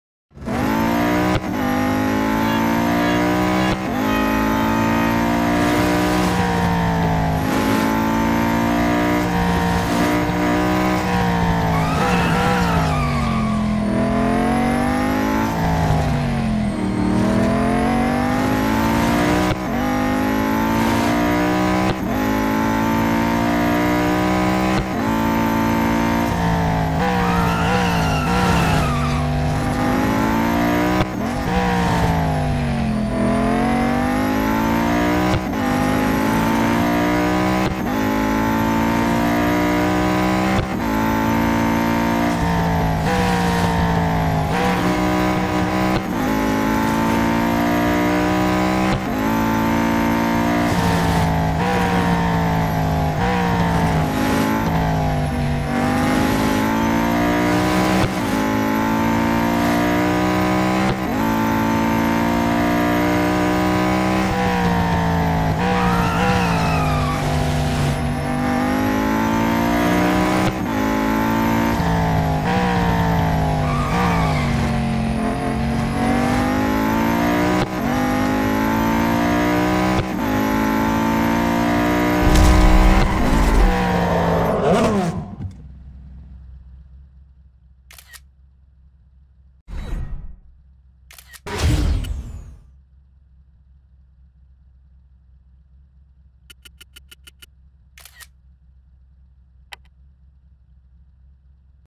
IMPROVED - WTT (Week 32, 2024) - Hennessey Venom F5 at Silverstone GP
Somehow I managed to improve on my previous time despite struggling with covid, in an eerily similar parallel to the Mercedes Straßenversion week in March 2023. It was a very good lap throughout, enough that I was able to get a solid P4 at the time despite having a slightly compromised exit out of Vale. To be fair, the lower speed allowed be to maximize every inch of track in the sweeping final corner, and the delta gave me a few hundredths. Maybe if I got Vale good I could've been P2, but given the circumstances, I can be more than satisfied with this.

Time: 1:18.087
Ranking: Upper Group A (#131, 81st consecutive, 4th as of Sunday)